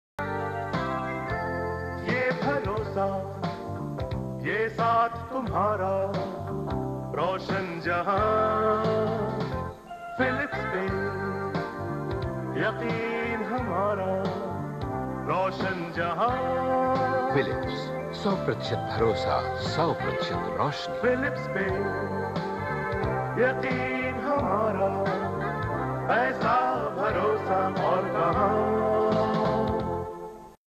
Philips advertisement India
Old 1990's advertisement of Philips Bulbs and tubes in India